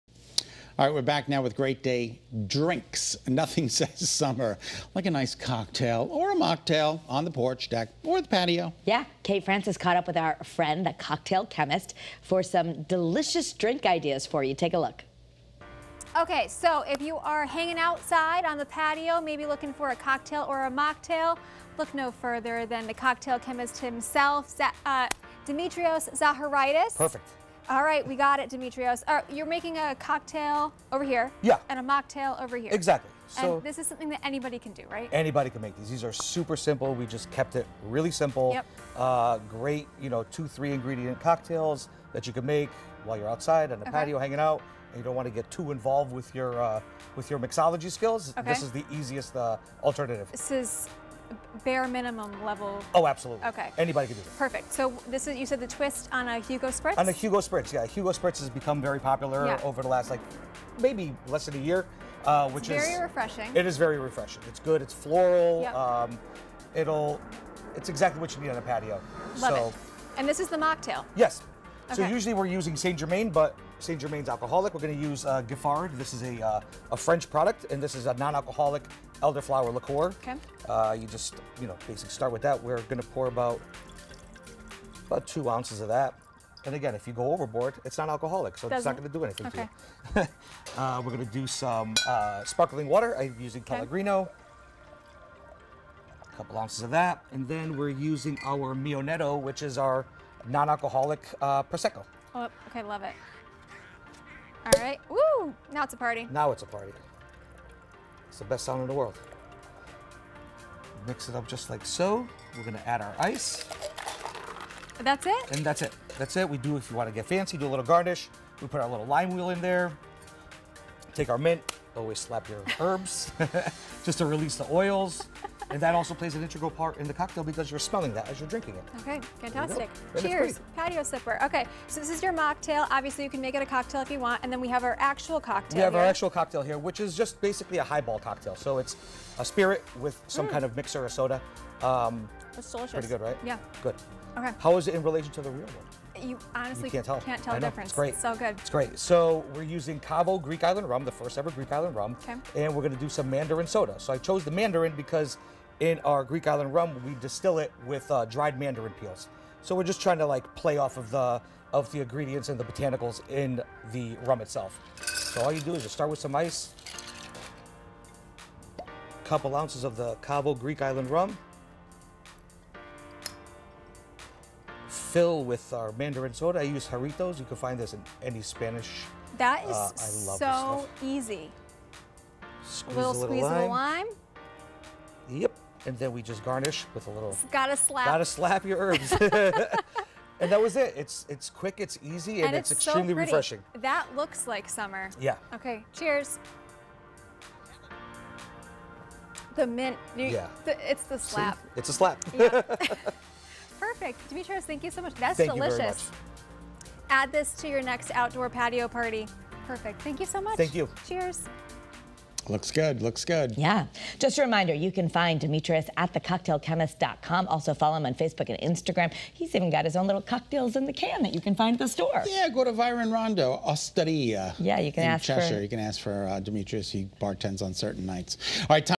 Great Day Drinks: Porch Sippers
The cocktail chemist shared some delicious drink ideas for a cocktail or mocktail to sip on your porch!